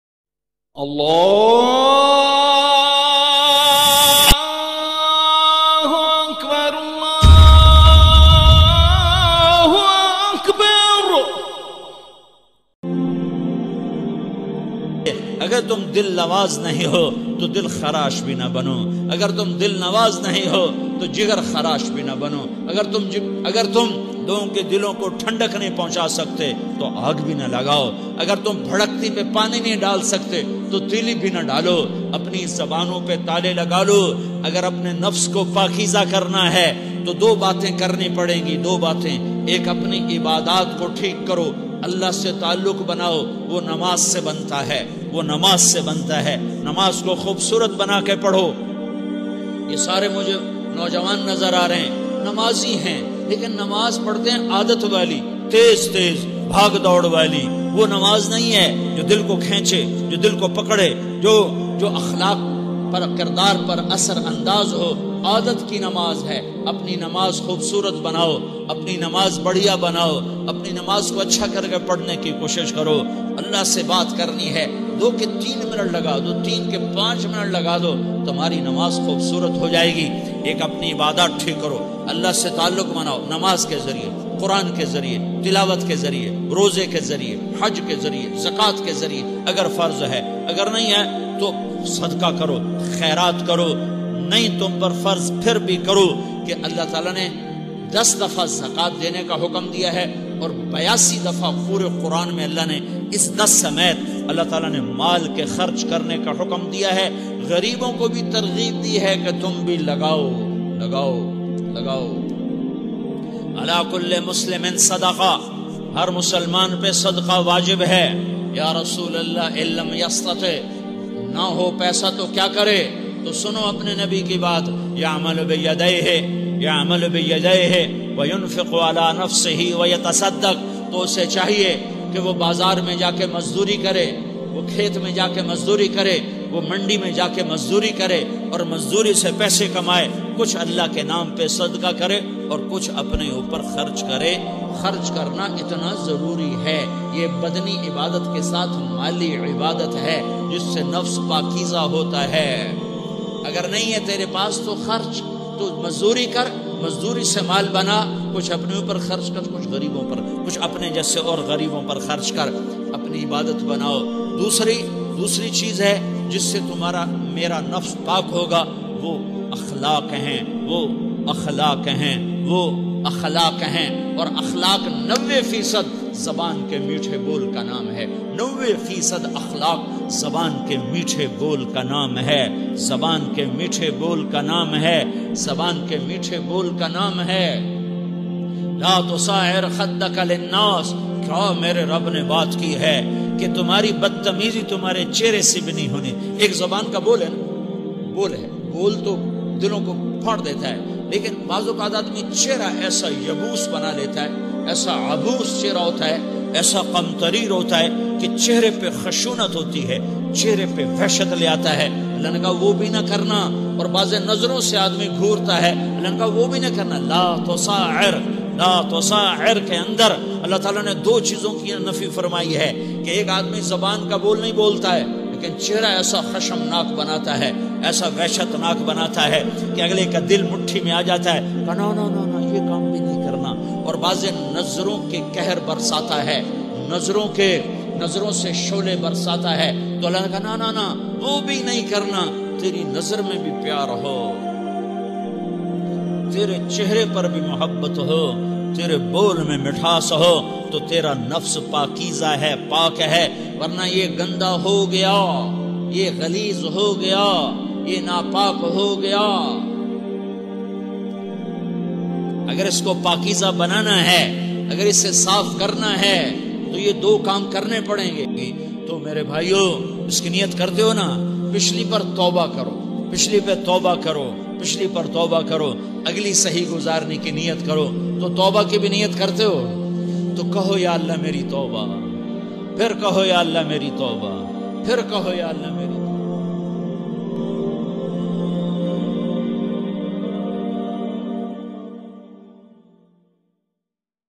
[ NEW SHORT CLIP ]_KISI KA DIL MAT TORO 💔_ MAULANA TARIQ JAMEEL SAHAB
Kisi ka dil mat toro _ Maulana Tariq Jameel Sahab

Topic :- KISI KA DIL MAT TORO
SPEAKER :- MOULANA TARIQ JAMEEL SAHAB

_BEST ADVICE FOR LIFE CHANGING
1_JUBAN PE KABU RAKKHO
2_NAMAZ KO SAHI KARO
3_Kisika Dil Mat Toro